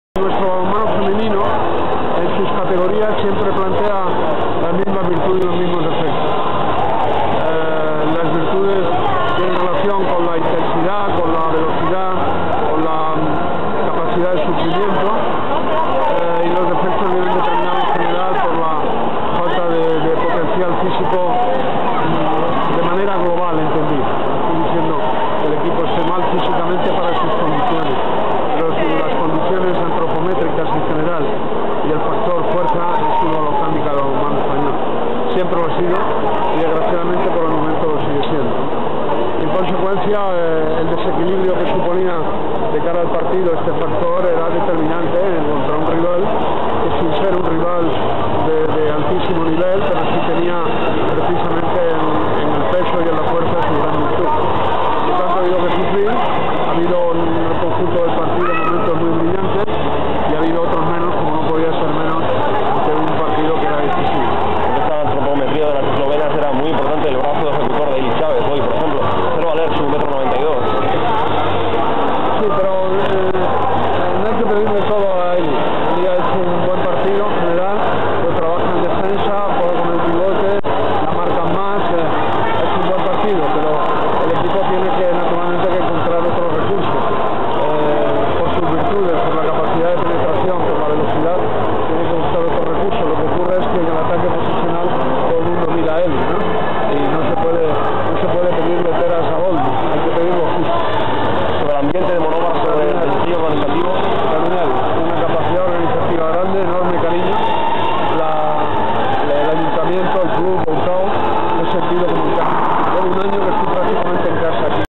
Juan de Dios Román.wmv
El primer mandatario de nuestro balonmano aborda las virtudes y defectos del femenino.